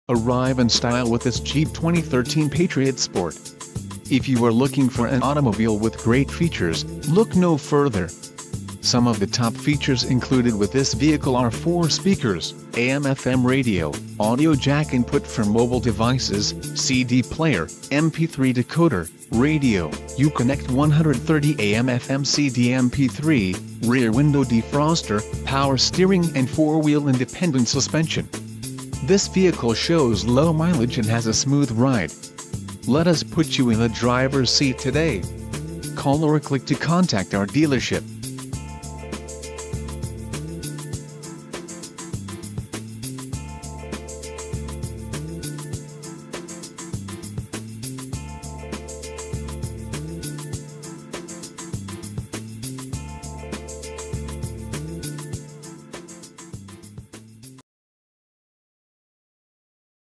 2013 Jeep Patriot Sport
Certified. Silver 2013 Jeep Patriot Sport FWD 5-Speed 2.0L I4 DOHC 16V Dual VVT Chrysler Group Certified Pre-Owned Certified, **LOCAL TRADE**, **CLEAN CARFAX**, **STILL UNDER FACTORY WARRANTY**, **FACTORY CERTIFIED 100,000 MILE WARRANTY!!**.

Chrysler Group Certified Pre-Owned Details:

  * Vehicle History
  * Roadside Assistance
  * Warranty Deductible: $100
  * 125 Point Inspection
  * Transferable Warranty